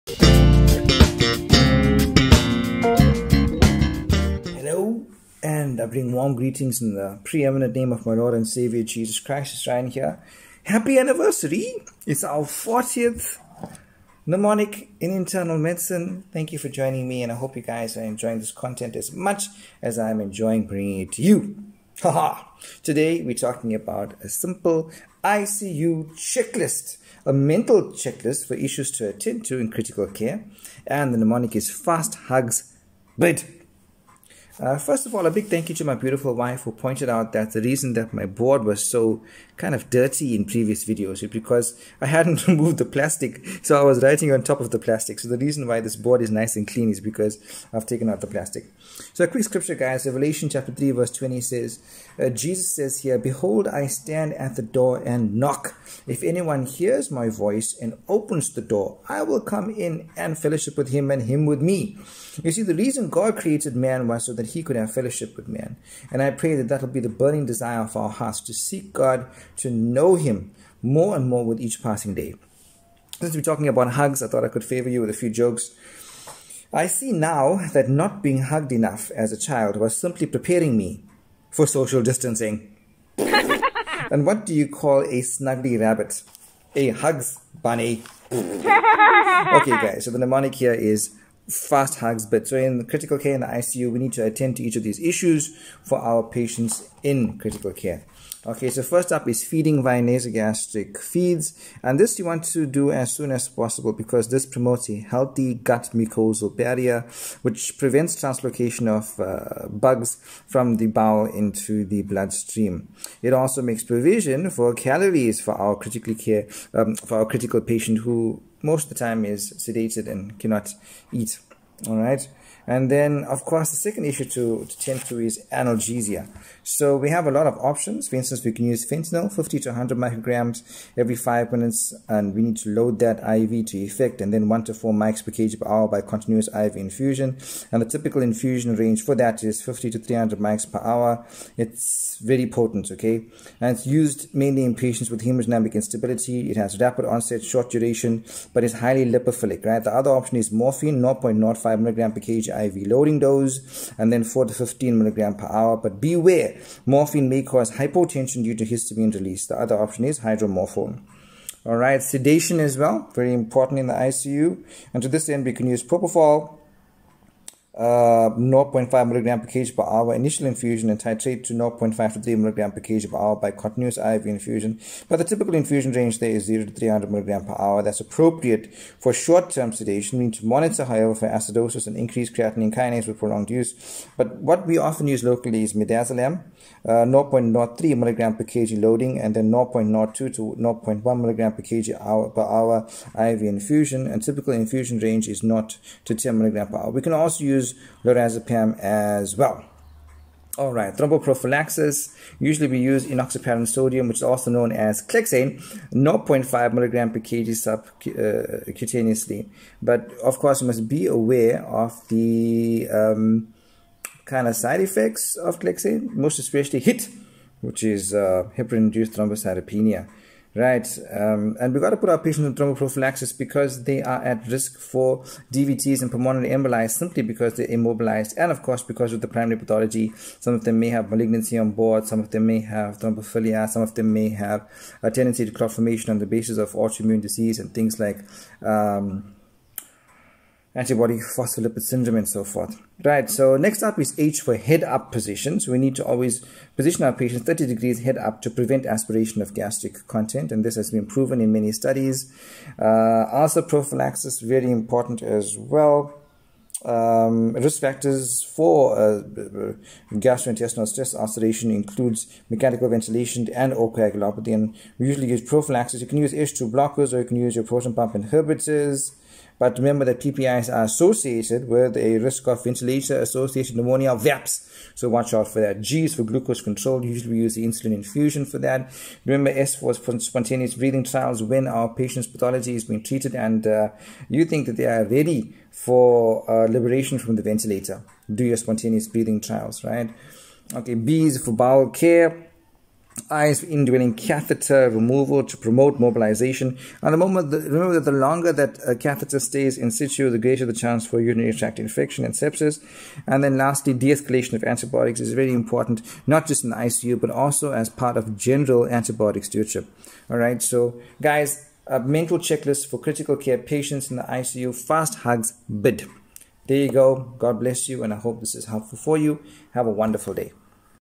Mnemonic 40 : ICU Care Checklist
Hi everyone. I bring warm greetings in the precious name of Jesus

Welcome to our 40th mnemonic in internal medicine!
Quite a common one today 'FASTHUGS BID' -a simple aid memoir for important points in ICU care

God bless
I trust you find this helpful
Stay safe!